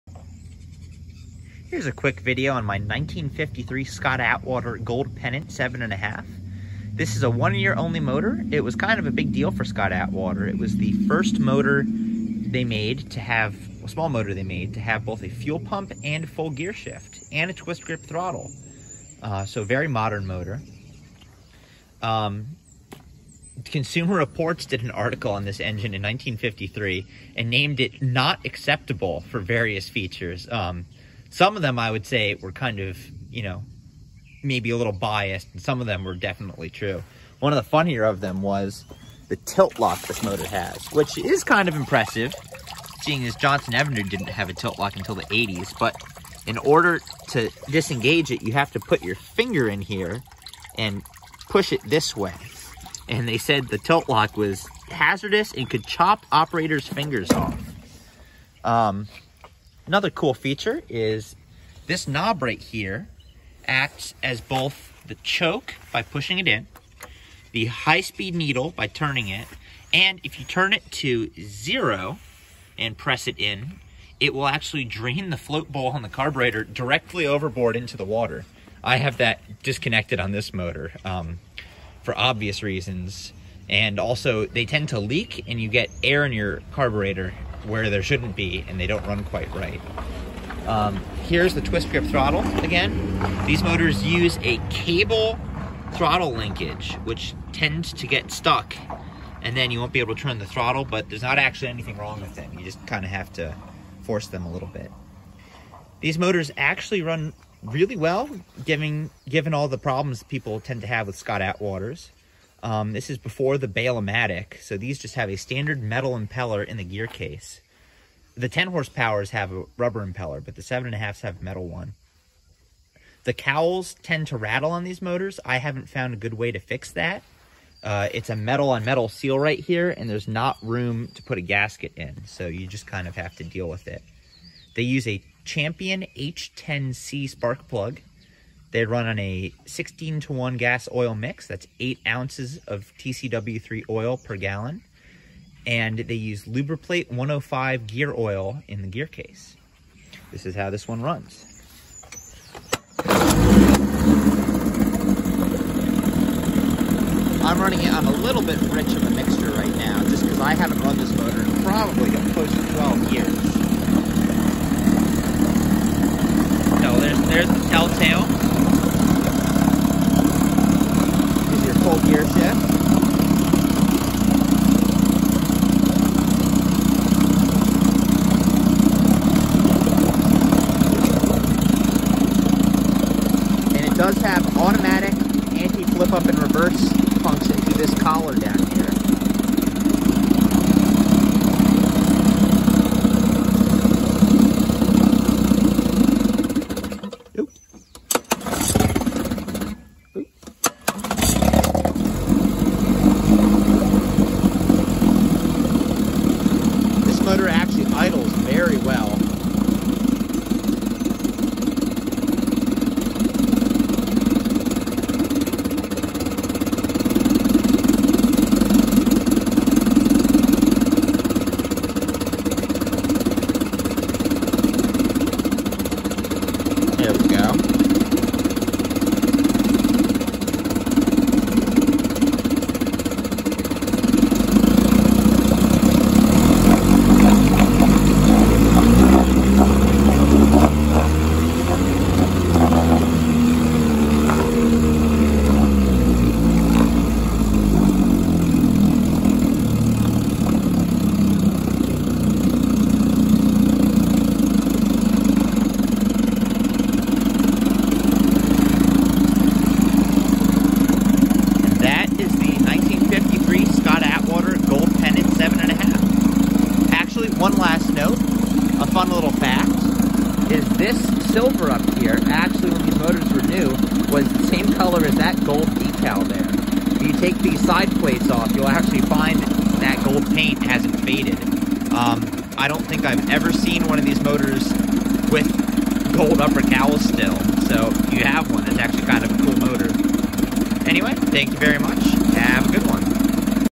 1953 Scott-Atwater 7.5hp “Gold Pennant” outboard
Here’s a quick video I threw together on my Scott Atwater Gold Pennant 7.5. This motor is one of my absolute favorites, and has never left me stranded.

I was given it probably 12 years ago, at which point in time it was in pretty rough mechanical shape. It was seized, the shift linkage was stuck, and it was on its way to the scrapyard. With a little tinkering, and a lot of PB Blaster, she makes a pretty great little fishing motor!

The Gold Pennant in my opinion, marks the dawn of the modern outboard motor. With a fuel pump, as opposed to OMC’s pressure tank, a twist grip throttle, and full gear shift, this motor brought to the table many of the conveniences we see as necessities now a days.

Sadly, the gold pennant was only a 1 year motor, only being produced in 53’. In 54’, Scott Atwater came out with the ill fated Bail-O-Matic system, which while cool on paper, more or less destroys the functionality of the rest of the engine.